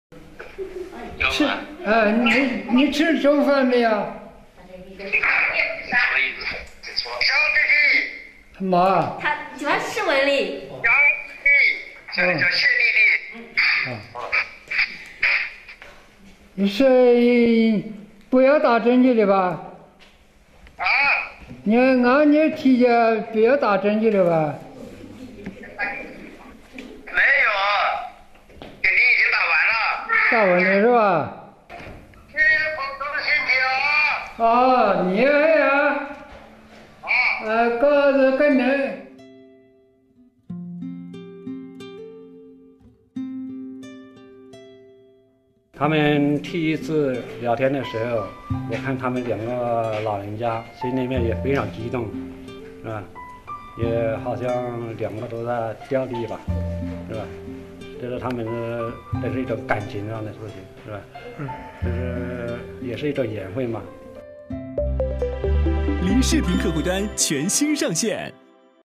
老兵61年后借视频重逢：含泪道珍重
4月3日，江西九旬志愿军老兵聂文华，通过视频终于见到阔别61年的老战友谢荣汉。几十年的战友情使他们一直记挂着对方，并最终通过网络联系上了。